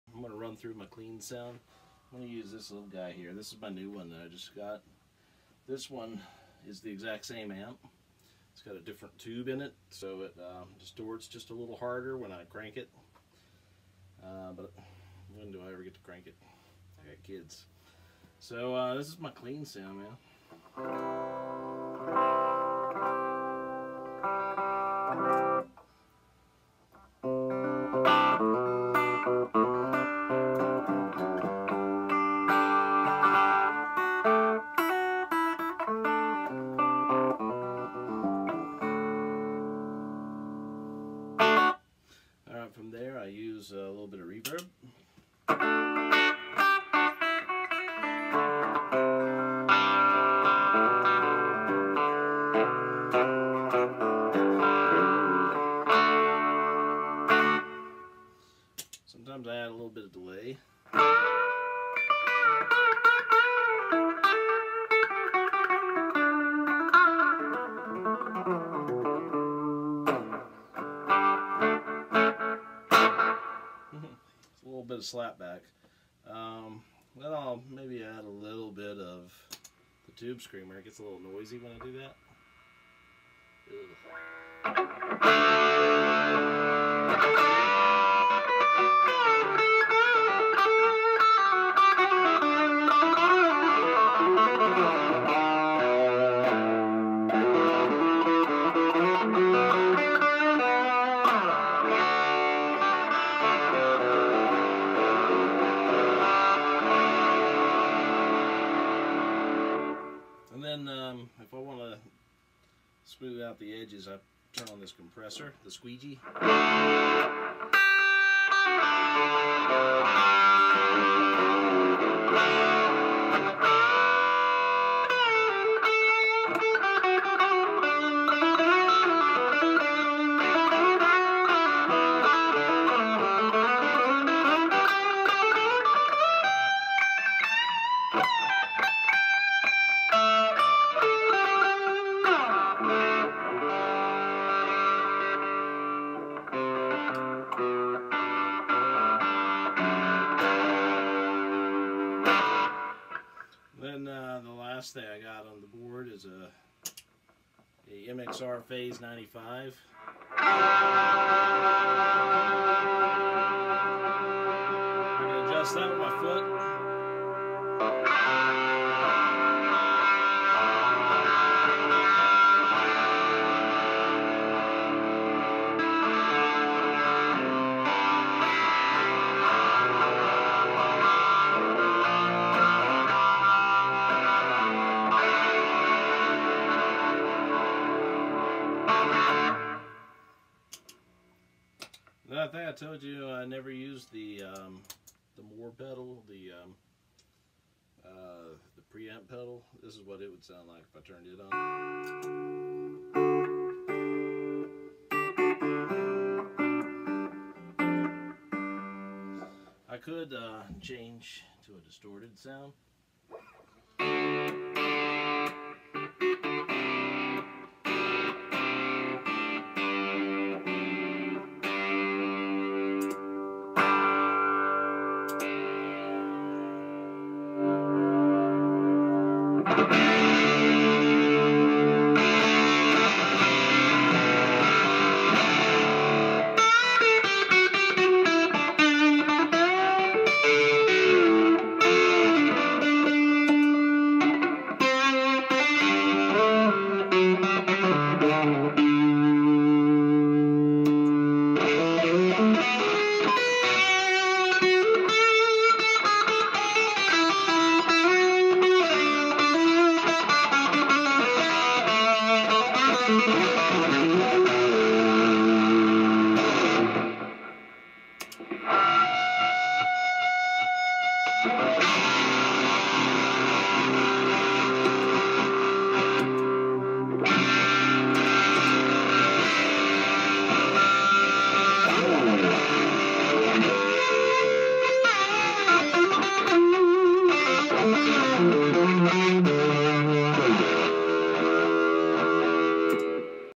Vox sounds and my Les Paul Studio plus top
Me goofing around with pedals.